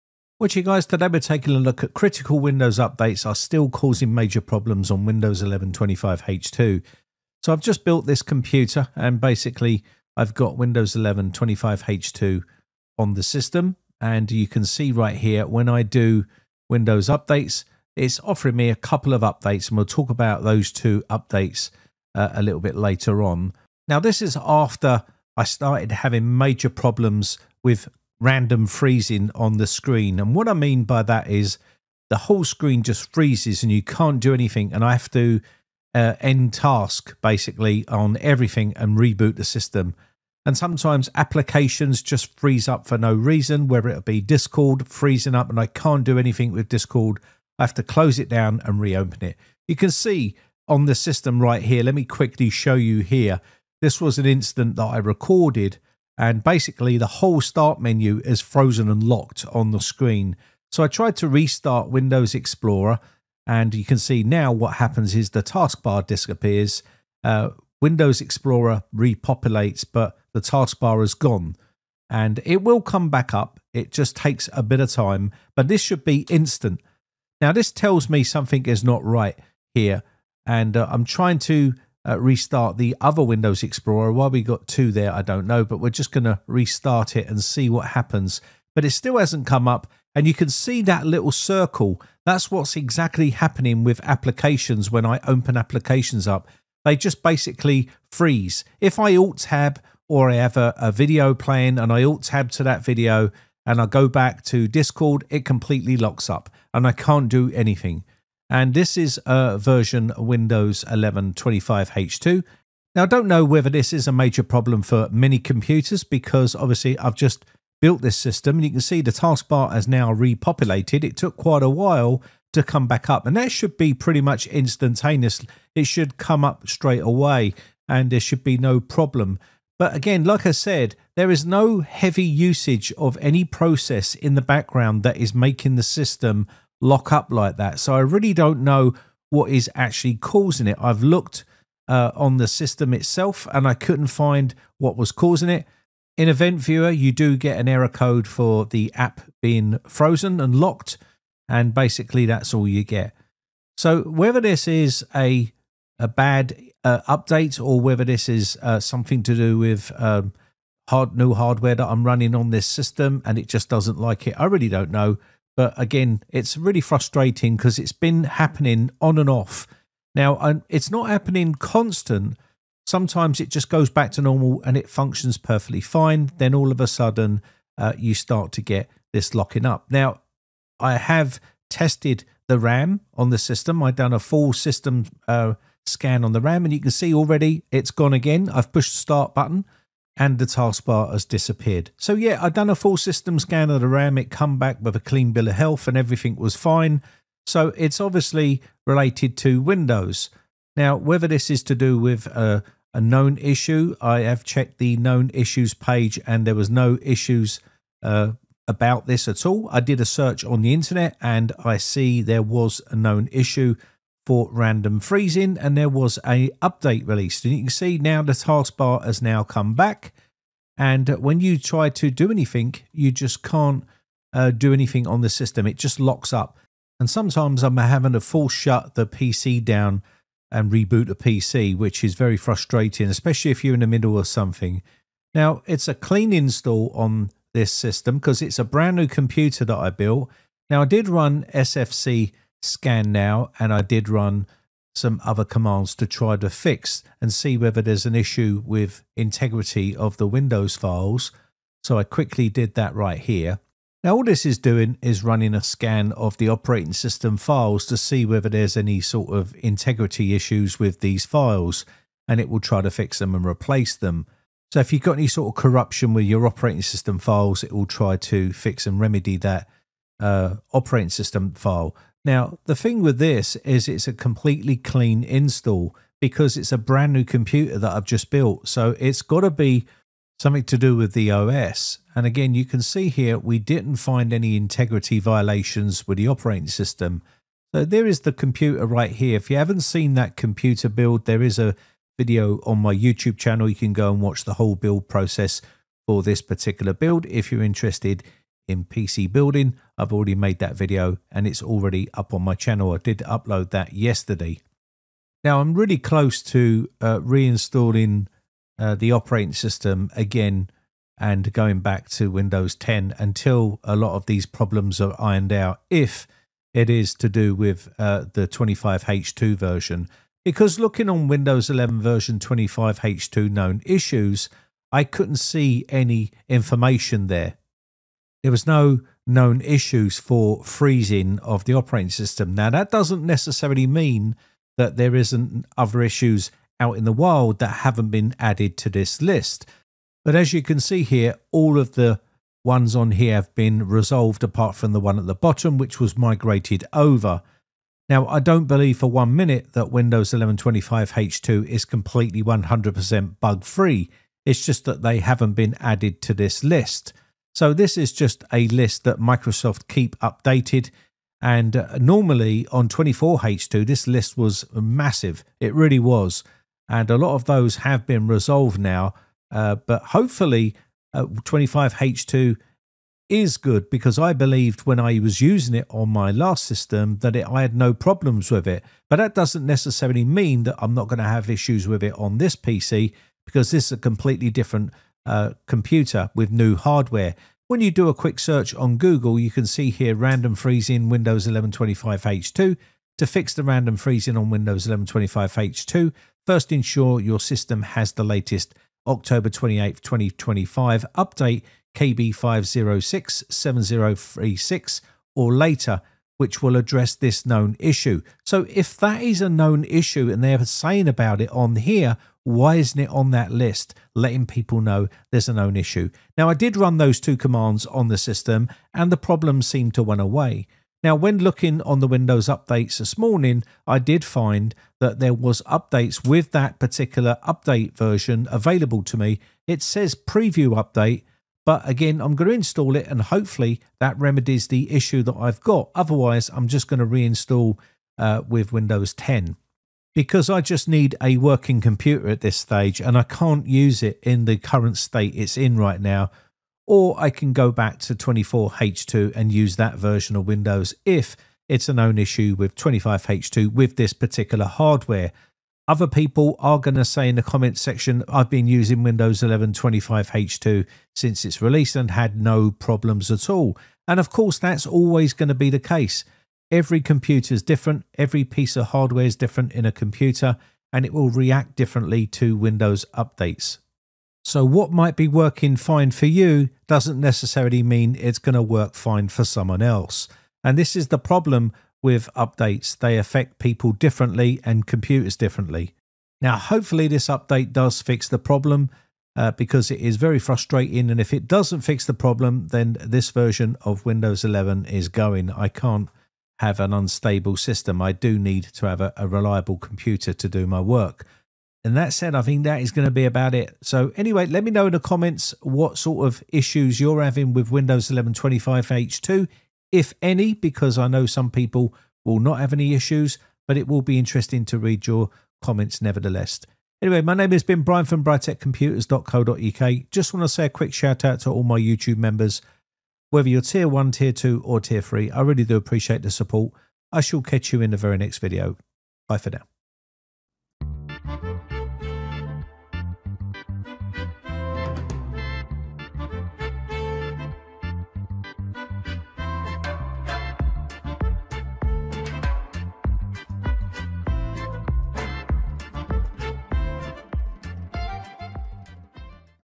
Critical Windows Updates Are Still Causing Major Problems
Major problem with Windows 11 25H2 where the computer keeps randomly freezing.